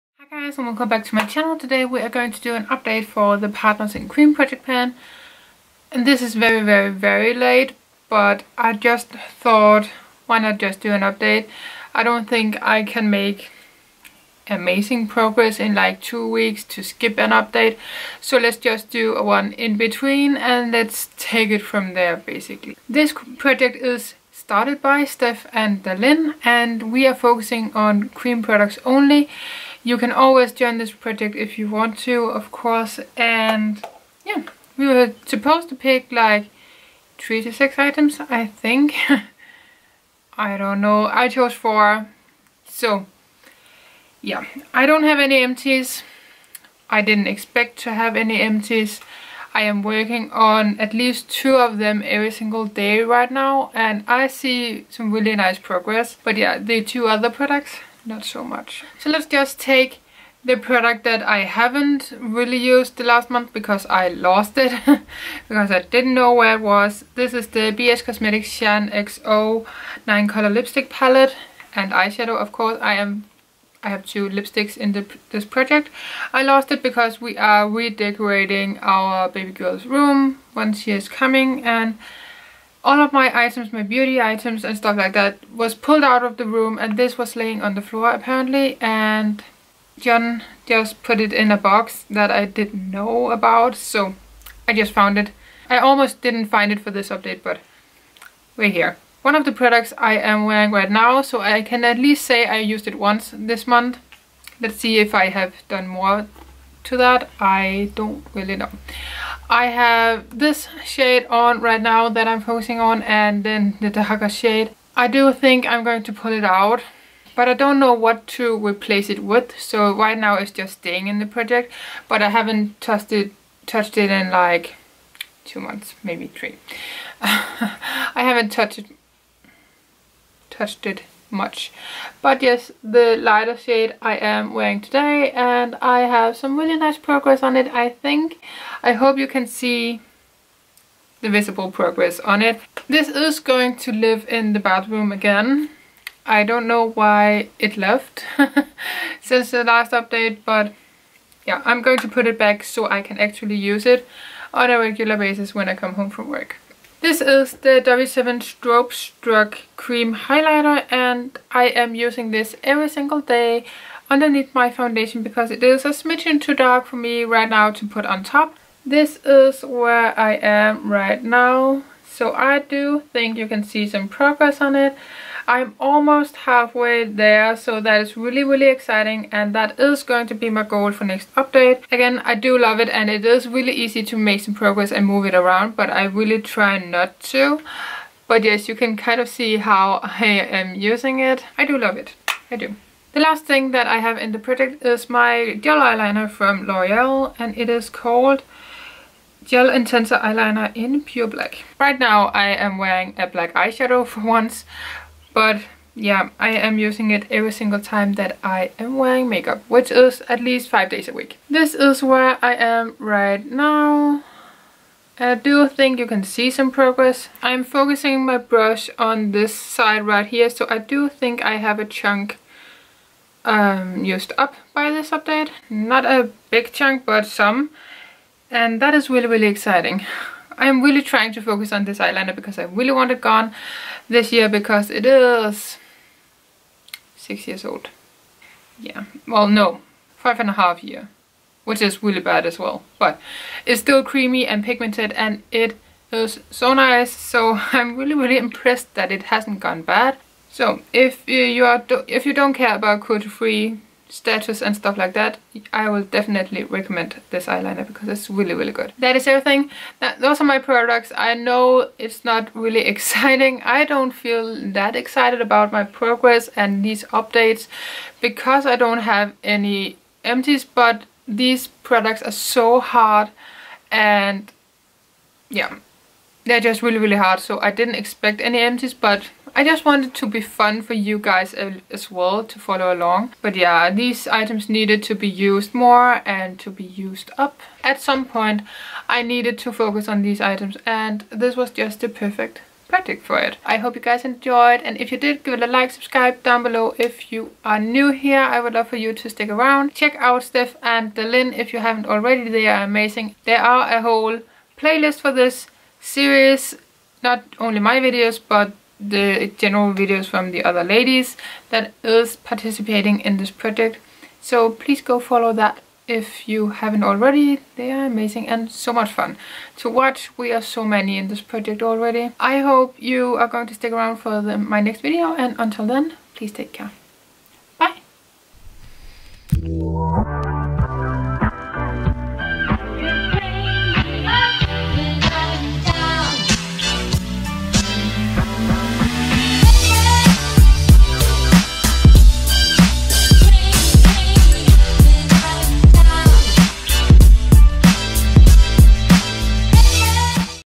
PARTNERS IN CREAM | UPDATE 4 // COLLAB
Start date: 10th January
End date: When ever you want
4-6 cream product - everything else than powder products
Monthly Updates
Goal can be hit pan or finish. You choose.

MY PRODUCTS:
 1) Loreal Gel eyeliner - Pure Black
 2) BH Cosmetics Shaaanxo palette - shade 7
 3) BH Cosmetics Shaaanxo palette - shade 9
 4) W7 Strobe Struck Highlighter*